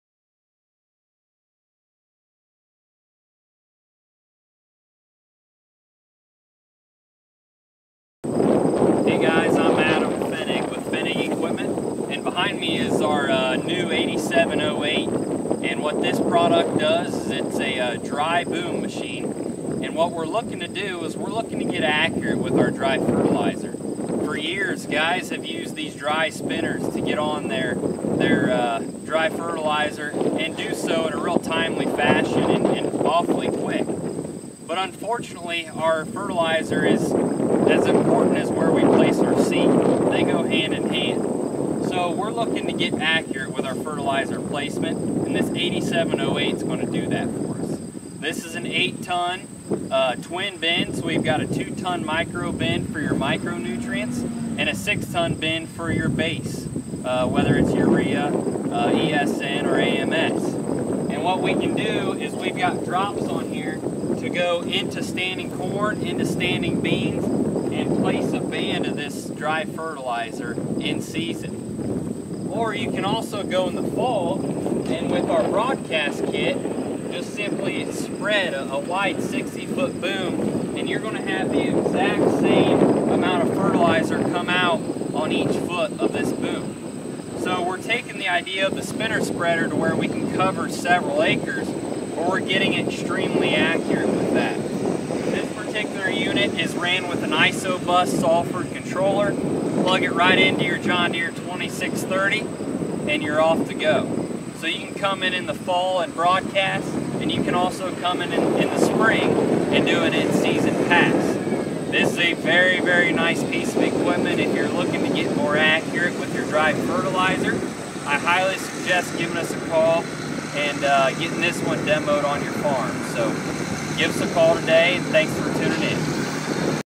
Valmar 8708 Air Boom Fennig Equipment
When it comes to dry fertilizer, it has always been a struggle to get accurate plaement while keeping efficiency high. Today, Valmar has changed the game placing fertilizer as accurate as 3% foot-foot across a 60 or 72' Boom! Spinner spreaders can cover acres but placement is extremely inconcistent causing yield loss throughout the year. This unit can also mount on John Deere High Clearance Sprayers!